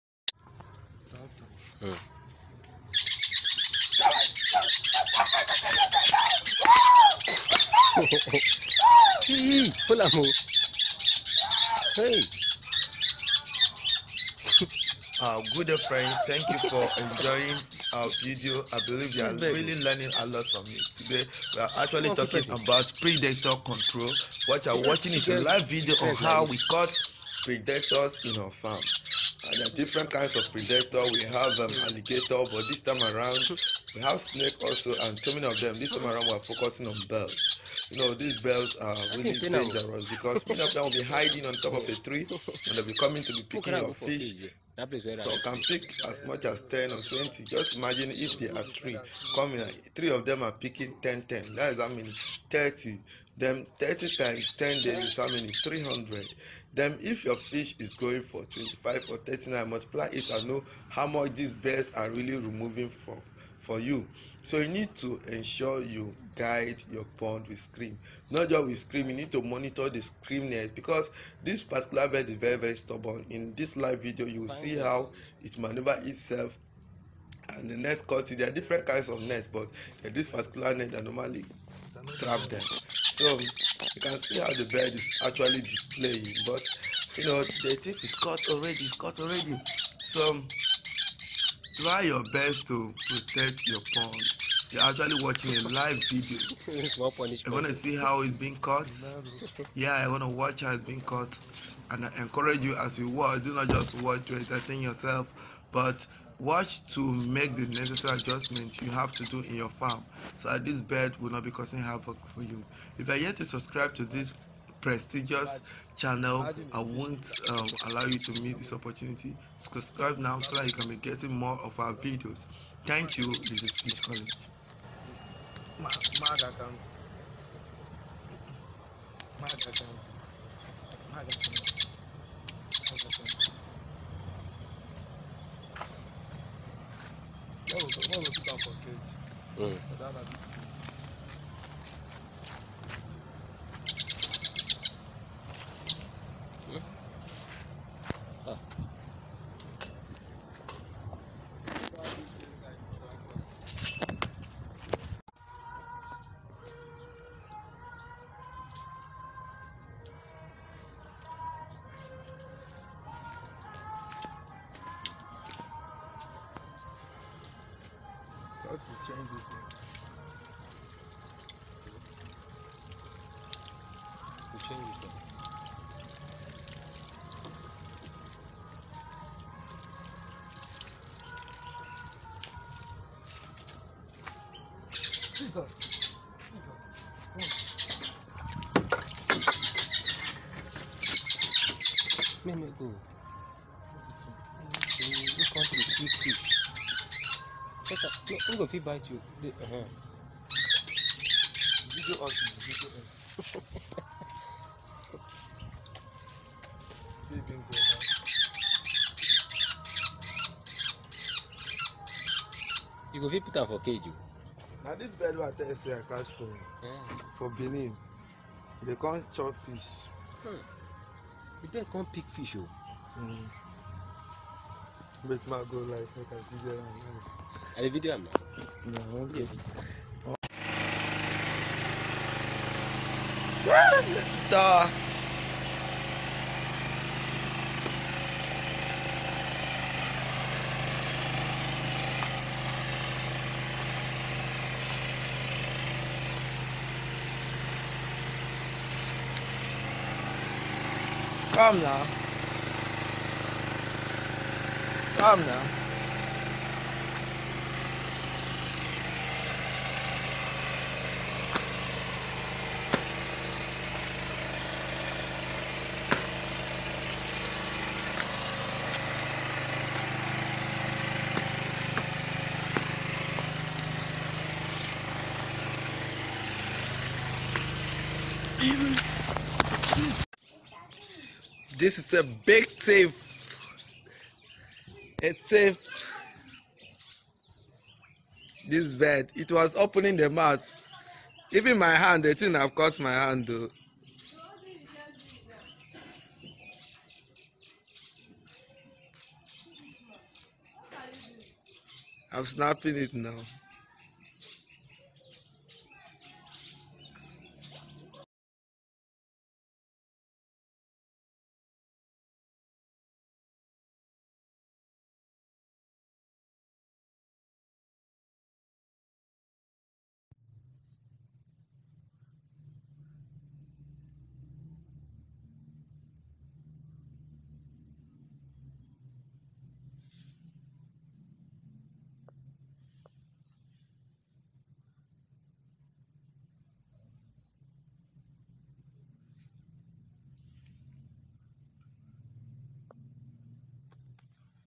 Fish College.Easy way to control predator (Bird) attack on fish.
Birds,snake, alligator,etc are common predator that reduce the quantity of fish in fish farm.This is live documentary on how to manage predator.